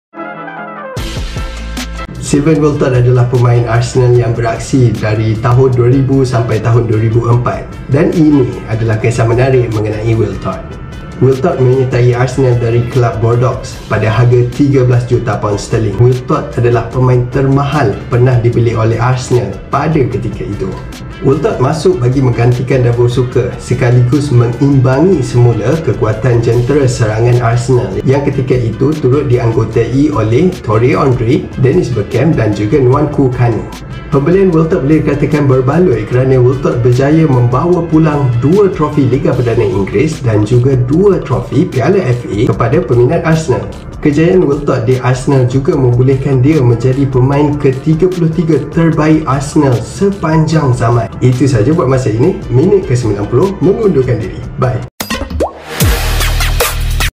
Minit Klasik Sylvian Wiltord
Ketika Arsenal membeli Sylvian Wiltord dengan harga £13 juta, dia turut mencipta sejarah menjadi pemain termahal kelab itu. Namun, dia membalas kepercayaan Arsene Wenger dengan membawa pulang 2 trofi Liga Perdana dan 2 trofi Piala FA.

Apa pula pulangan yang diberikan oleh Nicolas Pepe sejak menjadi pemain termahal dalam sejarah Arsenal?